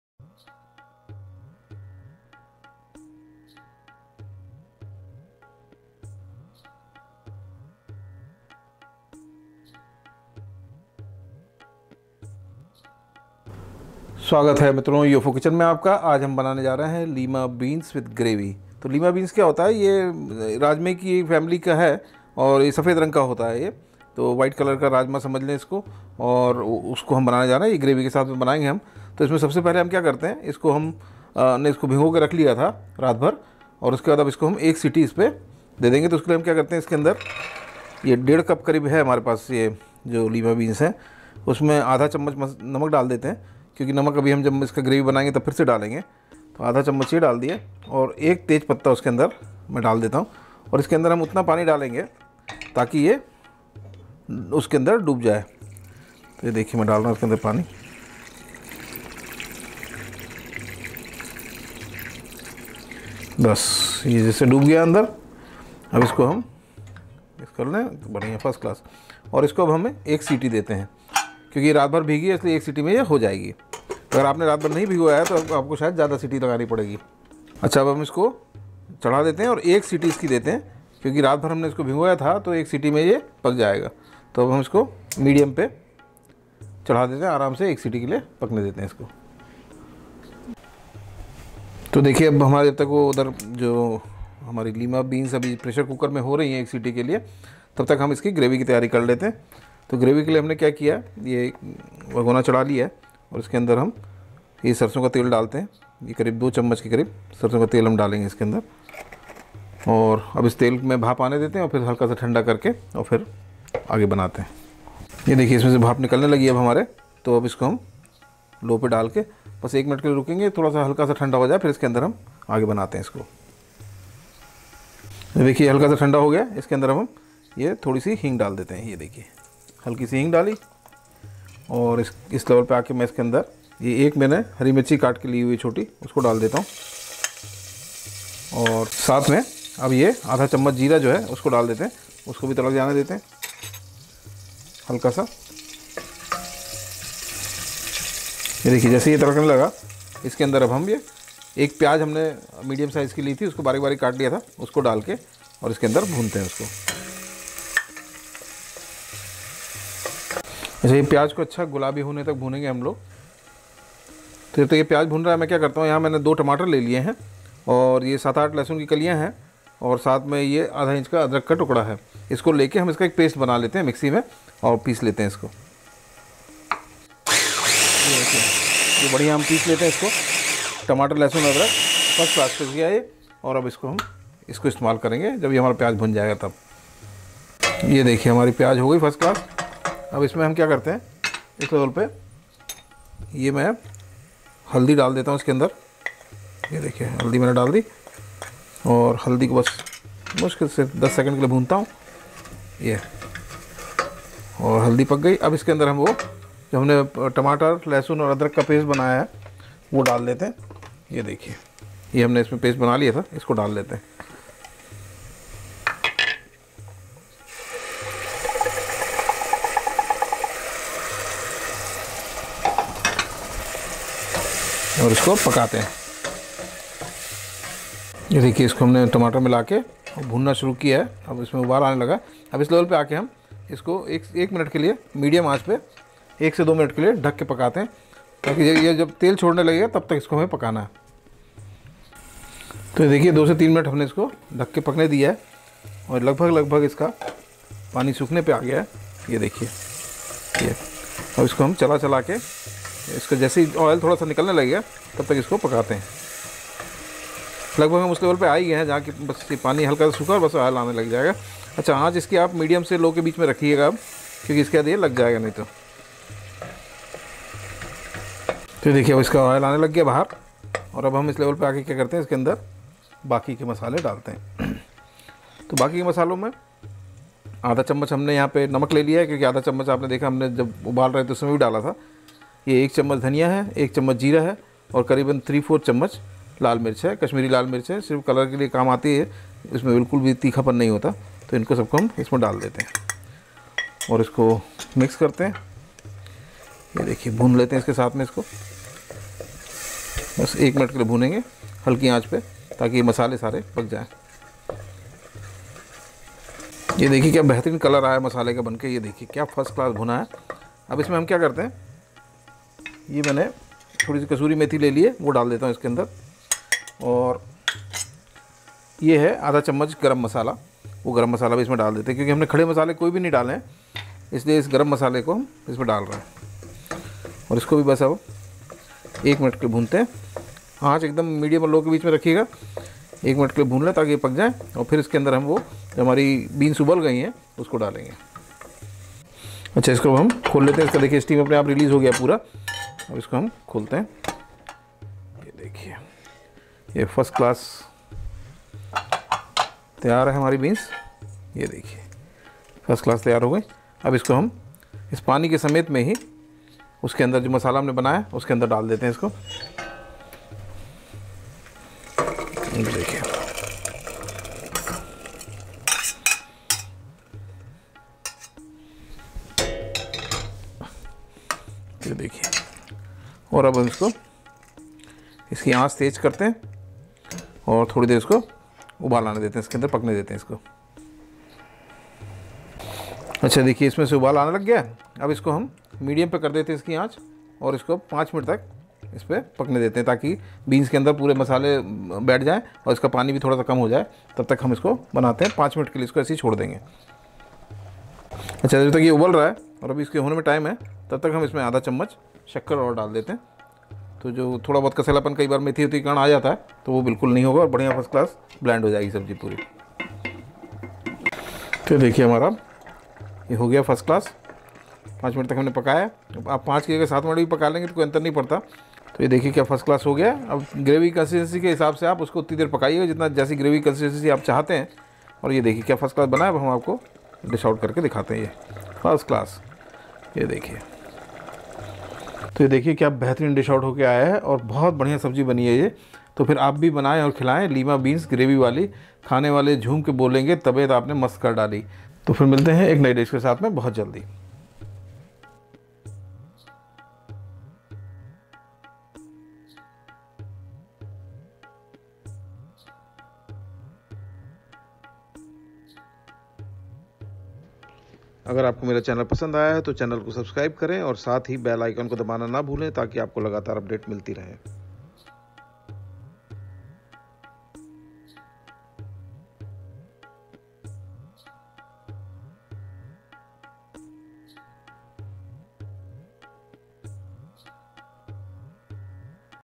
ऐसे बनाओ लीमा बीन, खाने का बदल जायेगा सीन Lima Beams Gravy Wali लीमा बीन्स ग्रेवी वाली
Lima Beans लीमा बीन्स is Mexican beans and taste really great. Here is the delicious Indian style for Lima Beans cooking:

Ingredients:
लीमा बीन्स – Lima Beans – 1 1/ 2 cup
नमक – Salt – 1 Tea Spoon
तेज पत्ता – Bay Leaves – 1
सरसों का तेल – Mustered Oil – 2 Tea Spoon
हींग- Asafetida – 1 Pinch
हरी मिर्च -  Green Chilli – 1
जीरा - Cumin Seeds – 1 /2 Tea Spoon
प्याज – Onion – 1 Medium
टमाटर – Tomato - 2
लहसुन – Garlic – 8 Cloves
अदरक – Ginger – 1/ 2 Inch
हल्दी – Turmeric Powder – 1/ 2 Tea Spoon
कश्मीरी लाल मिर्च पावडर –Kashmiri Red Chilli Powder – 3/ 4 Tea Spoon
धनिया पावडर – Coriander Powder – 1 Tea Spoon
जीरा पावडर – Cumin Powder – 1 Tea Spoon
कसूरी मेथी – Fenugreek Leaves Dry –  1/ 2 Tea Spoon
गरम मसाला – Garam Masala – 1/ 2 Tea Spoon
शक्कर – Sugar – 1/ 2 Tea Spoon